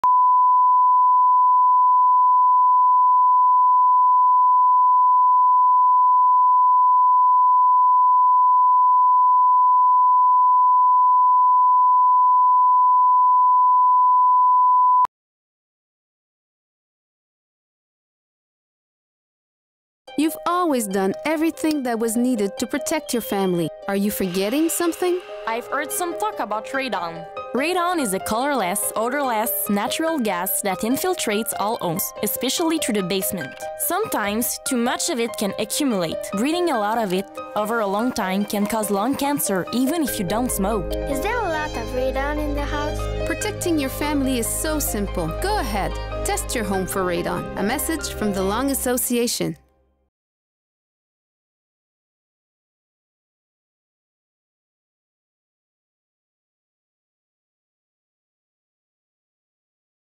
Radon - The 2nd cause of lung cancer after smoking
Radon is a radioactive gas produced naturally by the decay of uranium in the earths crust. The only known risk is long-term development of lung cancer. The level of risk depends on the concentration of radon as well as the number of years of exposure. It is estimated that 10% of all lung-cancer-related deaths in Quebec are linked to radon exposure.